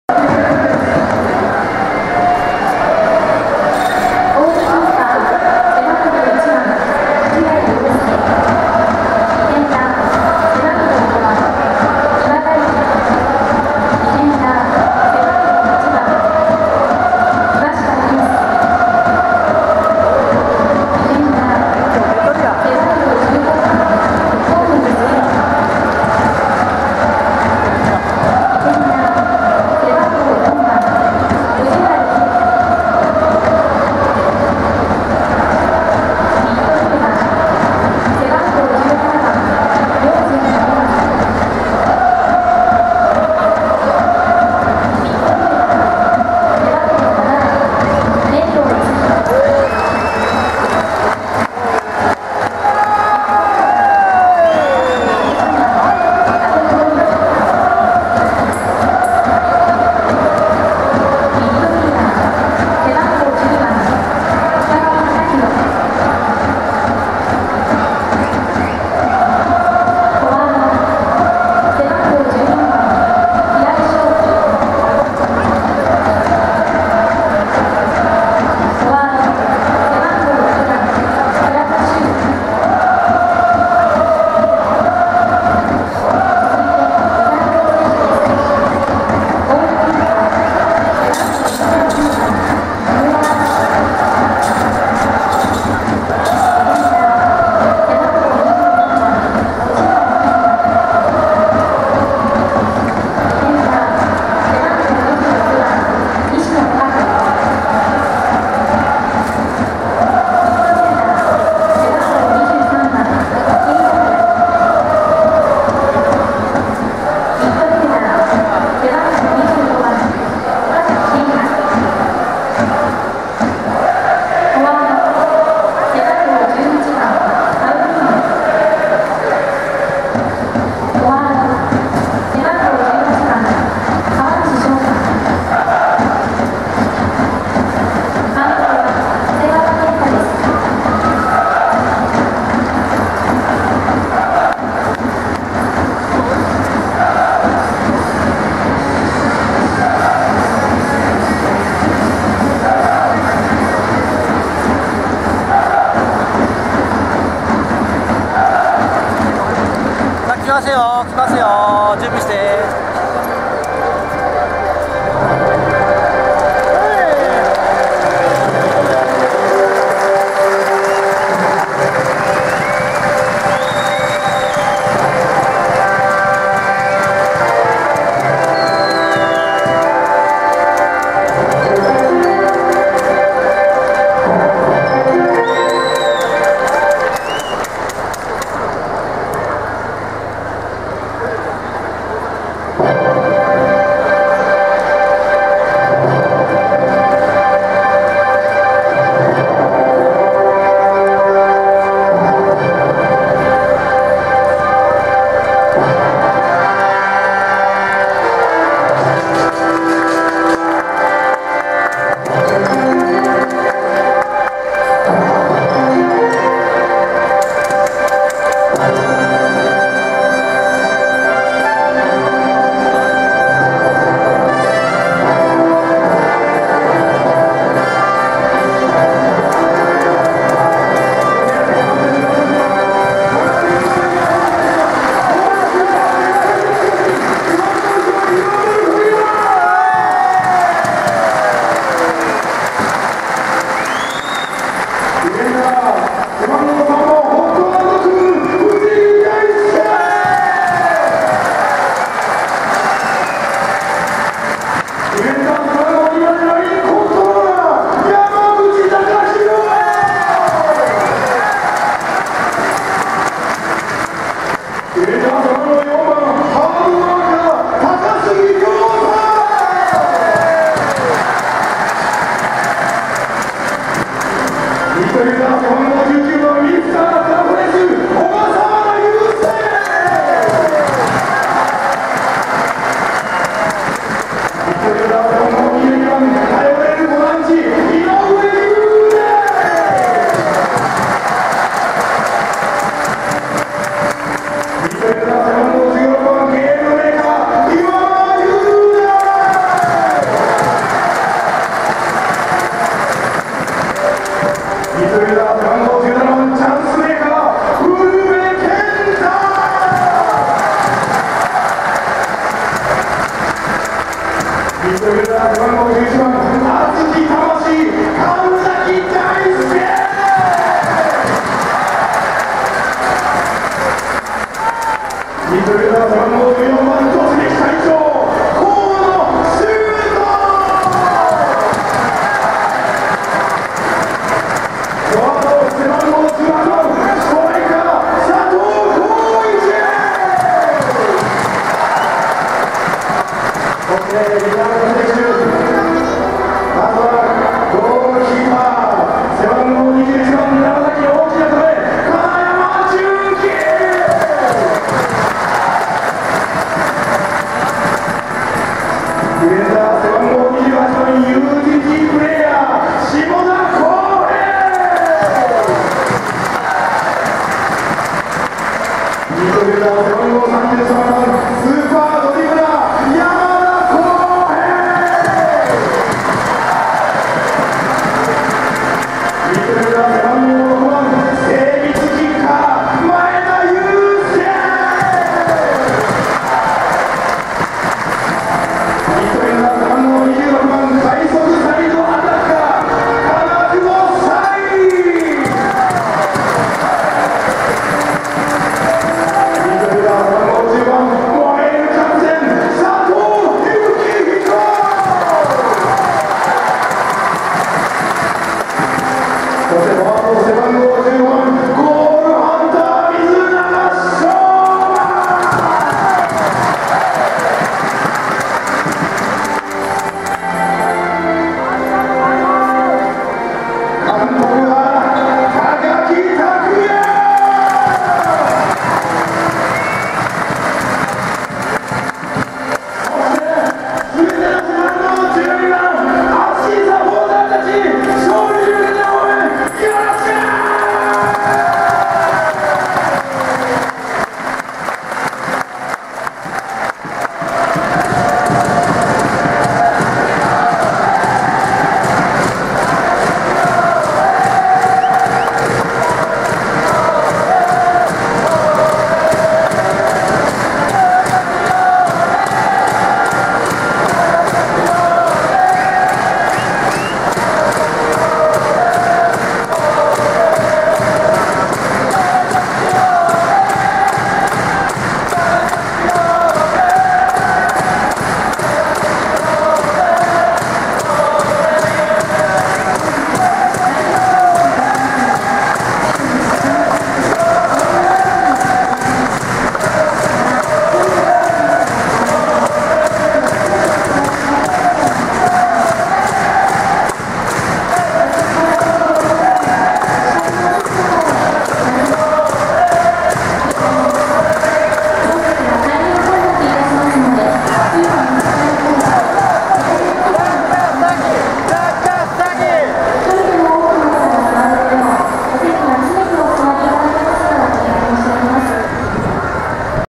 V・ファーレン長崎 vs ガンバ大阪 スタメン発表
2013年03月10日　12:35頃
ゴール裏コーナー付近より撮影